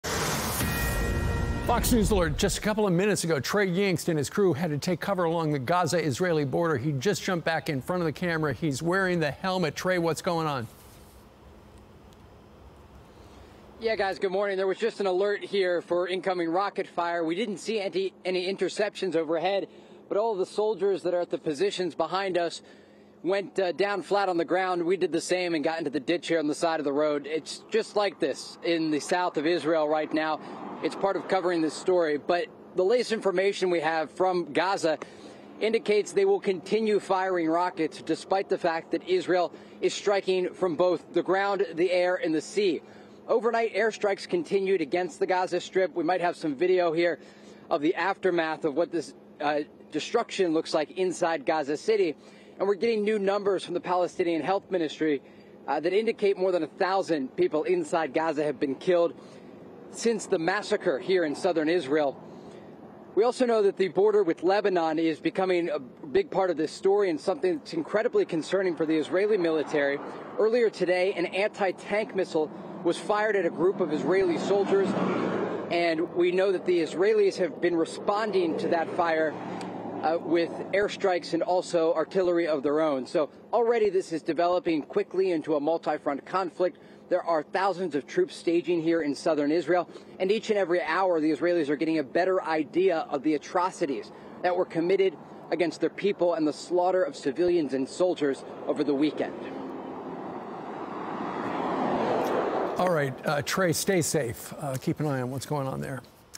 Trey Yingst forced to take cover in Israel as military shells Gaza
The Fox News crew was forced to take cover while reporting on the Israel-Gaza border as the war rages on.